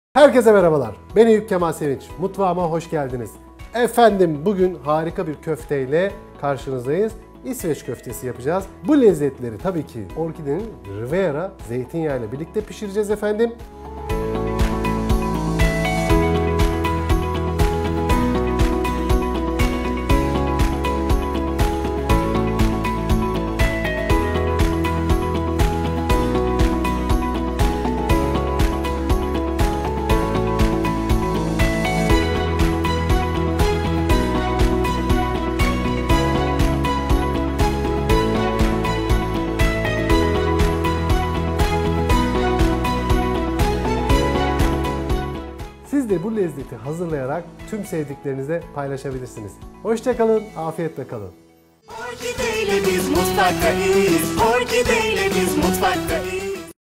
İSVEÇ KÖFTE | ŞEF EYÜP KEMAL SEVİNÇ
Orkide Lezzet Mutfağı'nda Şef Eyüp Kemal Sevinç ile İsveç Köfte hazırladık.

Orkide Zeytin Dalından Riviera Zeytinyağı'nın eşlik ettiği tarif; püf noktalarıyla birlikte Youtube kanalımızda.